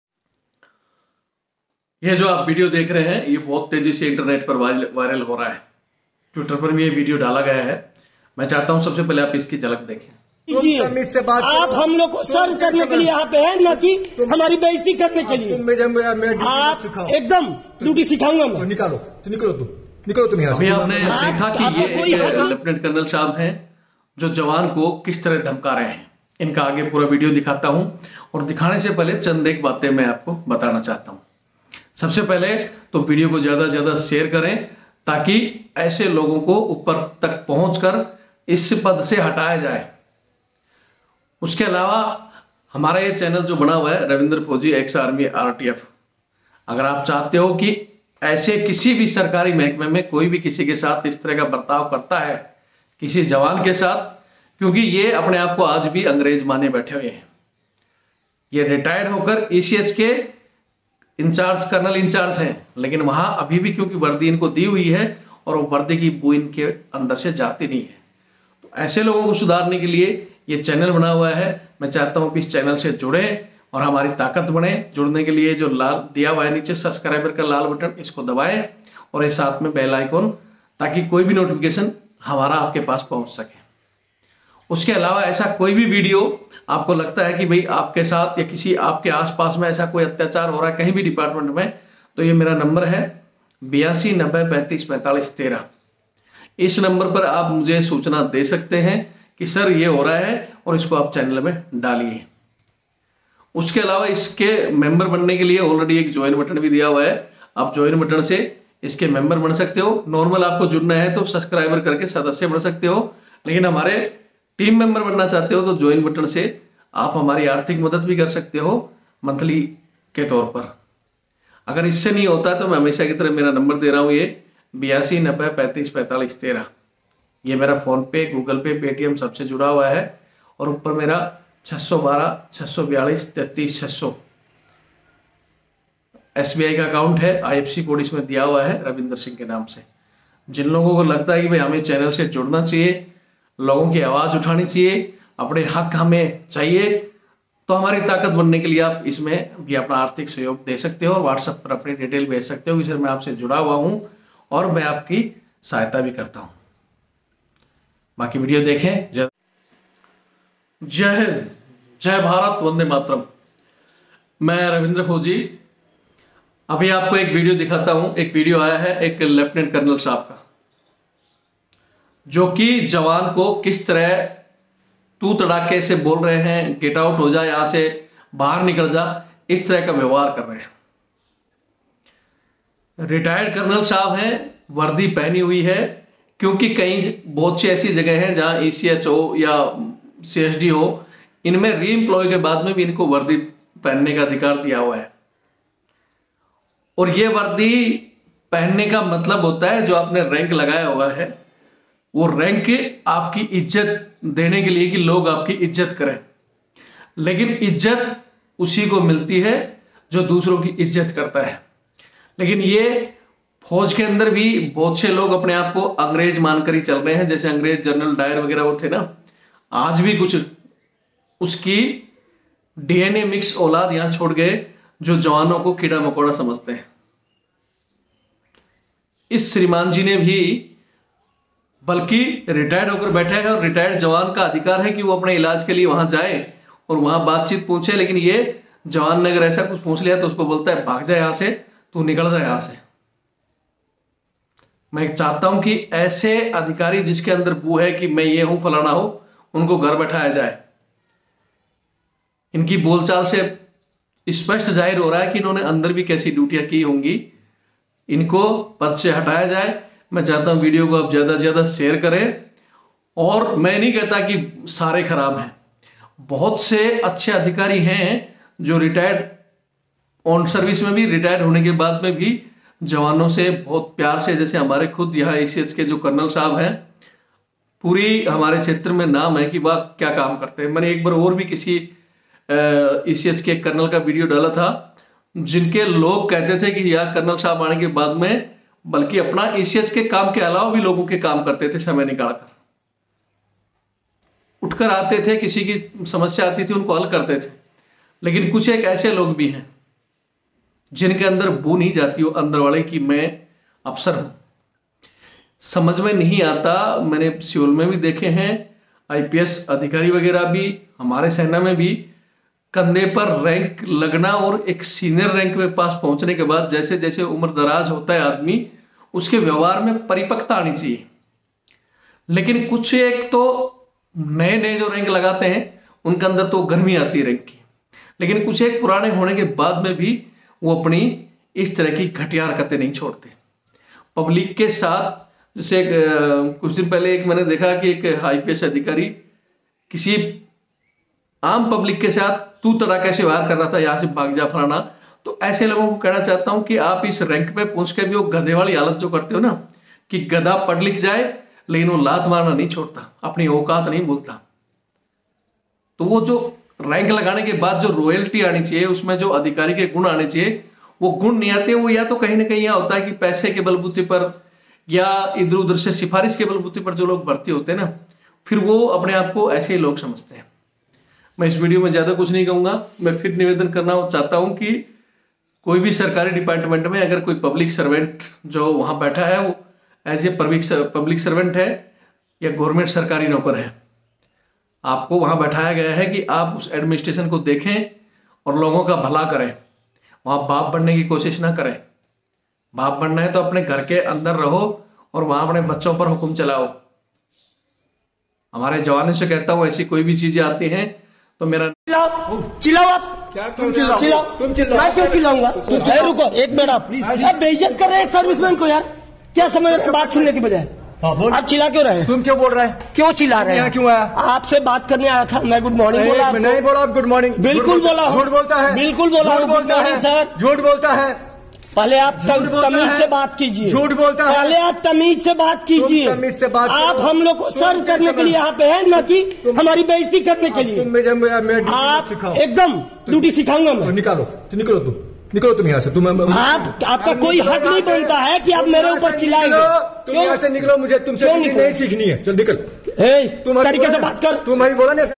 Lt COL ने हवलदार_को_दक्का_मारा ECSH में बोला निकल यहां से रविन्द्र फौजी ने CO से बोला
हवलदार ने बोला कर्नल को आप के बाप का नही ECSH मुझे लागू है और आप यहां के मालिक नहीं